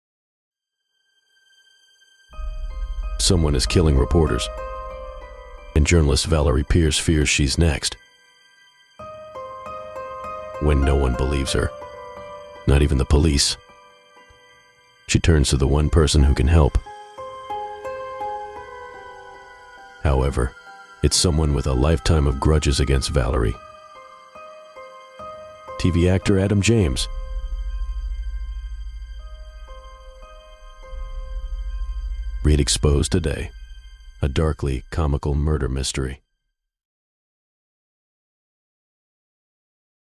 The darkly comical murder mystery novel Exposed, by Paul Ilett
Someone is killing reporters and journalist Valerie Pierce fears she is next. When no one will believe her, not even the police, Valerie sets out to catch the killer herself. But her plan involves teaming up with her arch nemesis – gay TV actor Adam Jaymes. The darkly comical murder mystery novel Exposed.

“Sensitive readers who do not enjoy murder and mayhem should not pick up this book”
☆☆☆☆☆ Kaivalya Khisti, The Online Bookclub US

"Exposed positions former-journalist Paul Ilett as one of the most exciting contemporary writers of the past few years."
☆☆☆☆☆ The Table Read Magazine